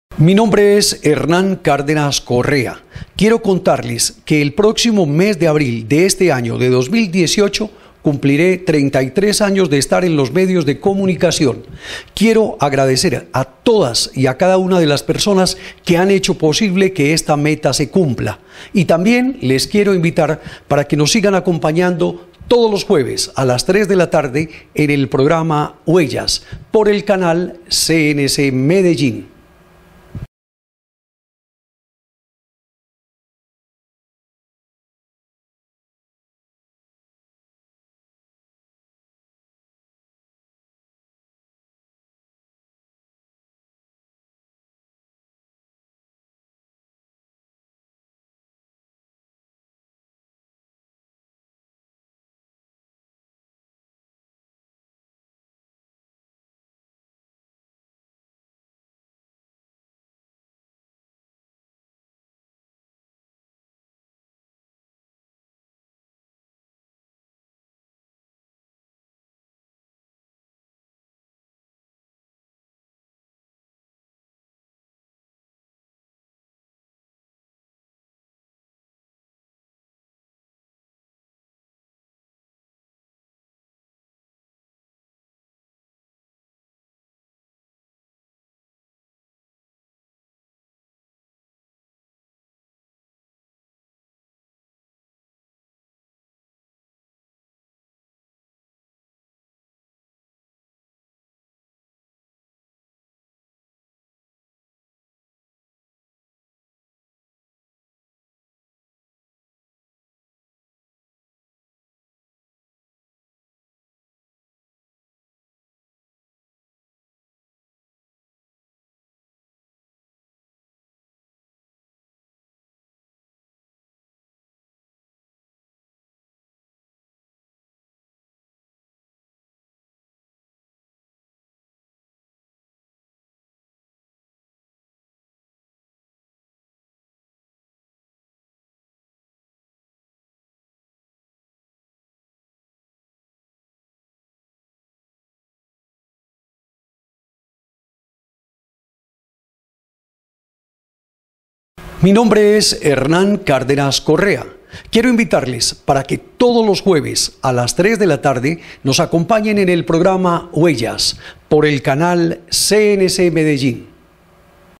Promo Hernàn Aniversario
En este mes de abril de 2.018 cumplirè 33 años en los Medios de Comunicaciòn